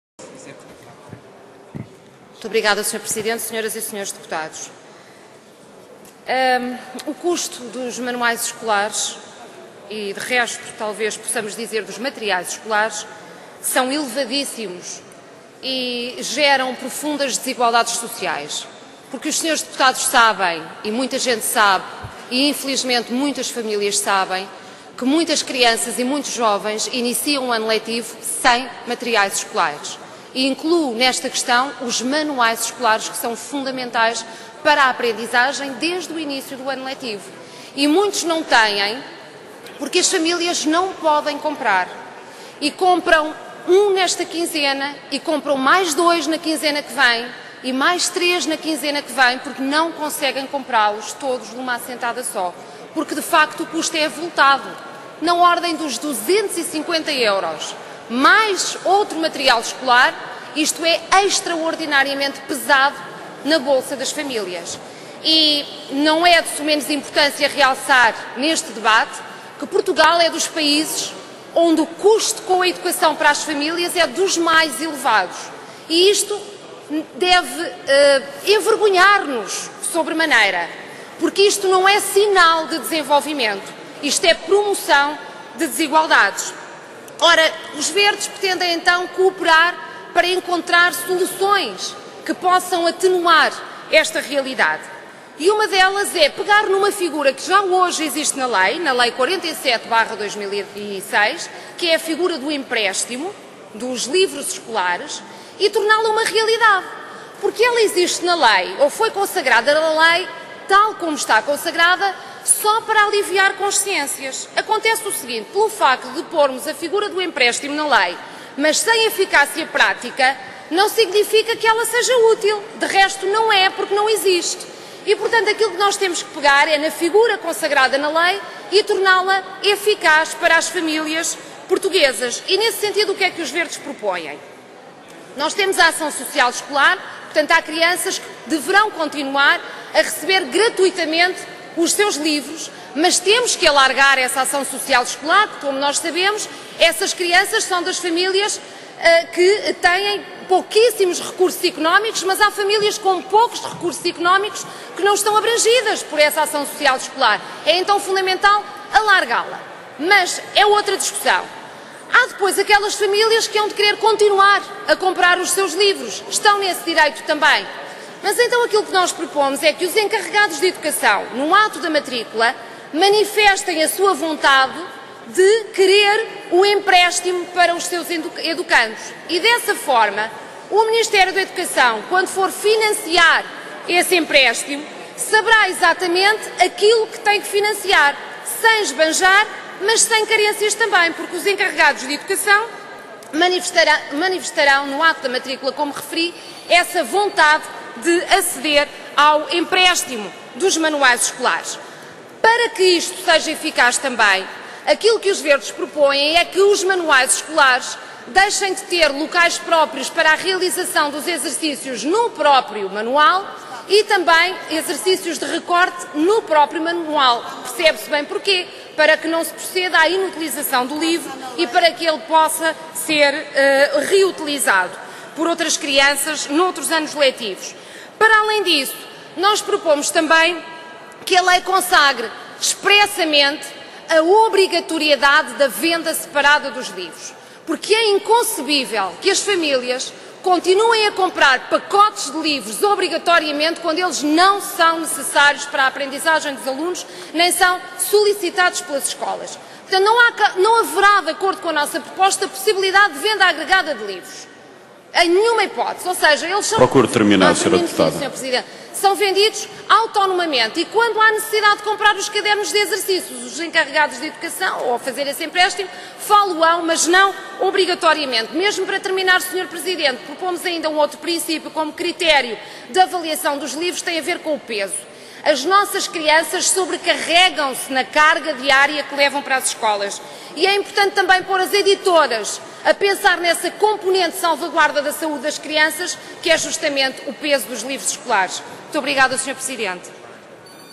Iniciativa do PEV - manuais ecolares
Apresentação do Projecto de Lei do PEV sobre empréstimo de manuais escolares - 1 de Outubro de 2010